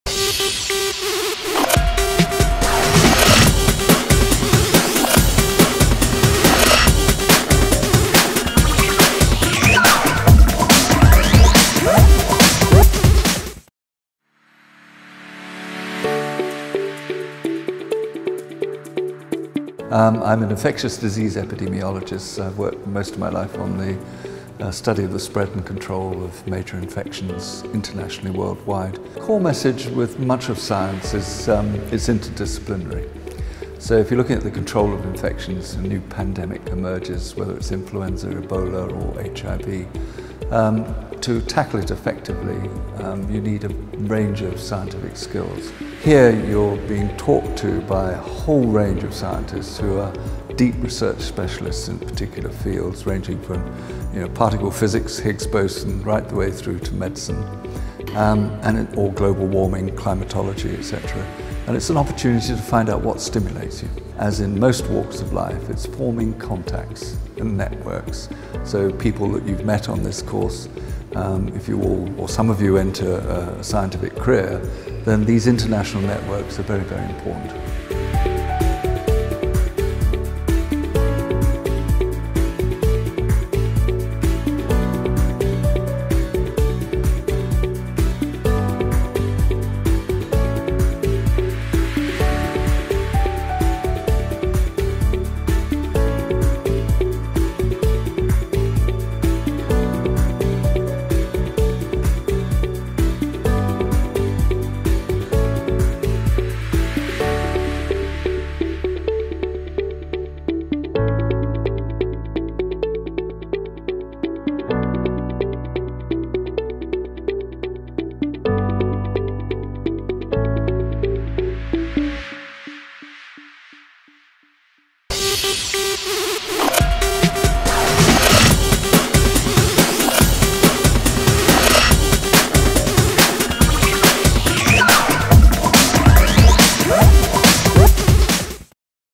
59th LIYSF STUDY DAY [OFFICIAL 2017 VIDEO]
LIYSF's Specialist Study Day was an interactive and informative study day for all participants to enjoy. With presentations, smaller lecture groups learning about more specialist parts of science and engineering, this day was great to show off and share the knowledge and talent of our participants.

Video Credit: Koray Turhan (LIYSF Videographer and Editor)